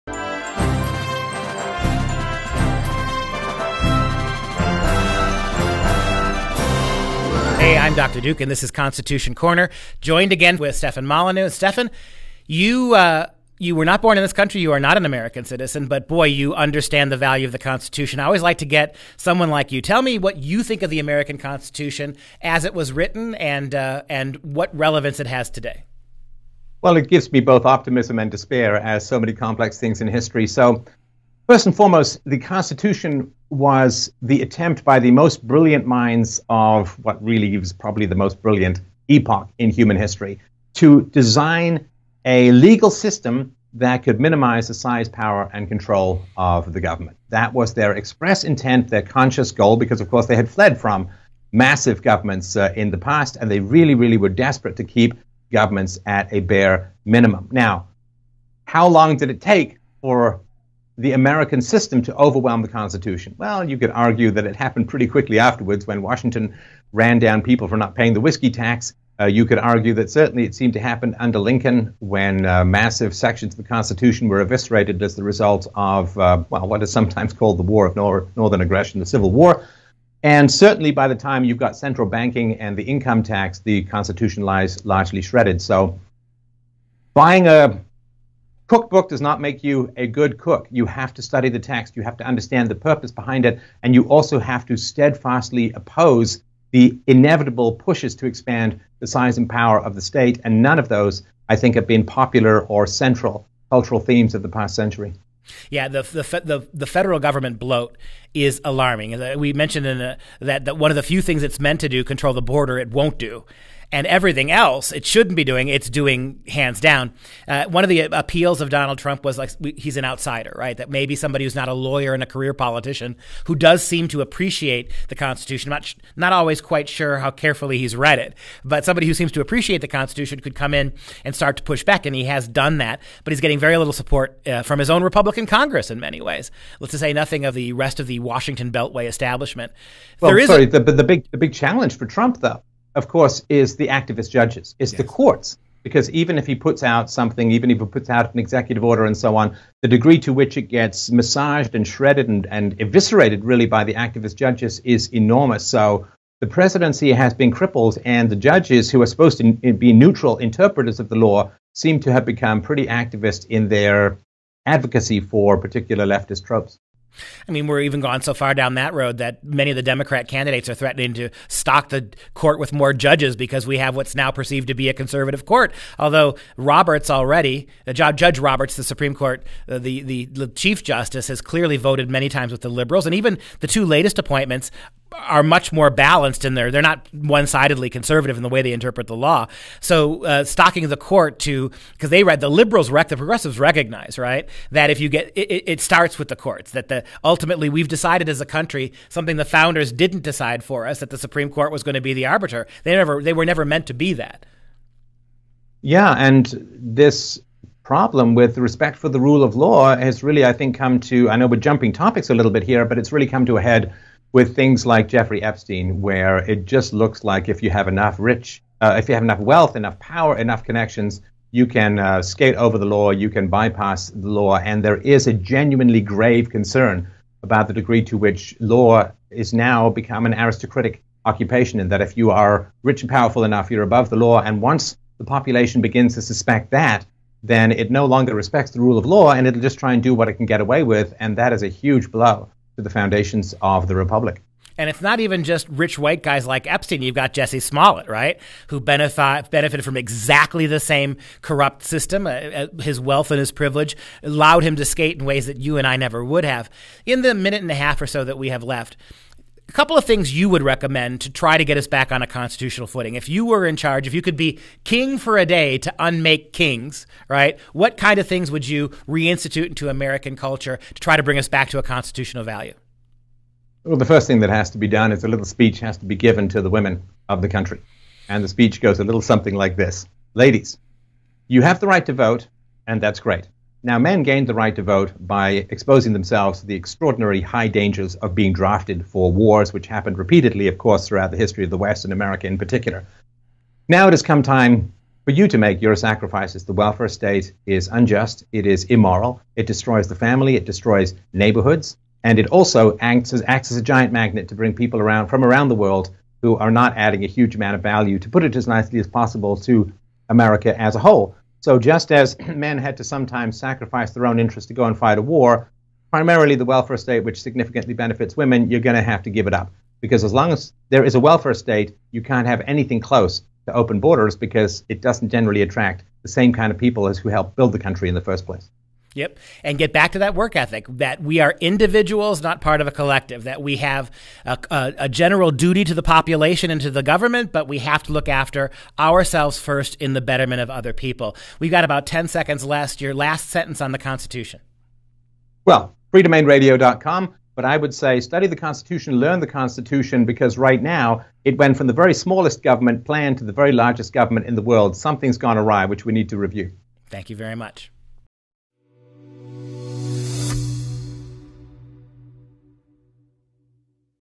Stefan Molyneux - How to Enforce the Constitution
Stefan Molyneux, founder and host of freedomainradio.com joins Dr. Duke Pesta to discuss the challenges facing the Constitution and how to get back to a constitutional republican government.

LIKE THE JOHN BIRCH SOCIETY AND WANT TO GET INVOLVED?